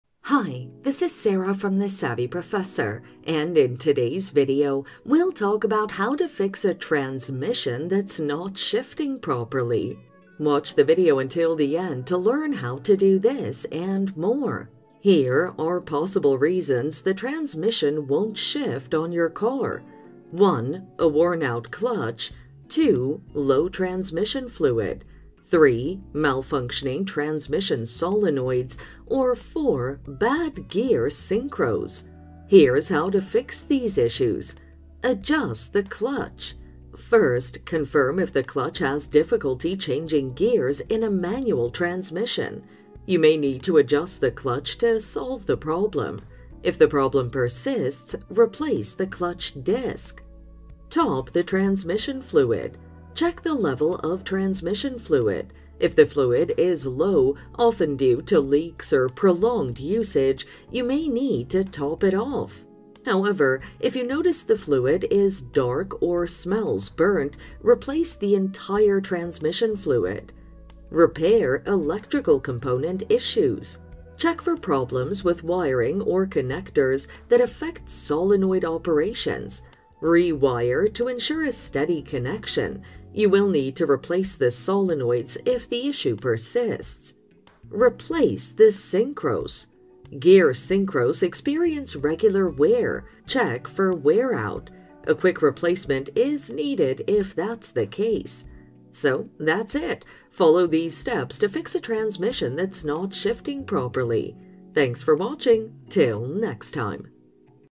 How To Fix Transmission Not Shifting Properly (Causes Why Transmission Won't Shift On & Solutions!)
How To Fix Transmission Not Shifting Properly (Causes Why Transmission Won't Shift On & Solutions!). In this video tutorial I will show you how to fix transmission not shifting properly.

Here are possible reasons the transmission won't shift on your car:
1. Worn-out clutch.
2. Low transmission fluid.
3. Malfunctioning transmission solenoids.
4. Bad gear synchros.

Adjust the clutch
• First, confirm if the clutch has difficulty changing gears in a manual transmission.
• You may need to adjust the clutch to solve the problem.
• If the problem persists, replace the clutch disc.

Top the transmission fluid
• Check the level of transmission fluid.
• If the fluid is low, often due to leaks or prolonged usage, you may need to top it off.
• However, if you notice the fluid is dark or smells burnt, replace the entire transmission fluid.

Repair electrical component issues
• Check for problems with wiring or connectors that affect solenoid operations.
• Re-wire to ensure a steady connection.
• You will need to replace the solenoids if the issue persists.

Replace the synchros
• Gear synchros experience regular wear; check for wear out.
• A quick replacement is needed if that’s the case.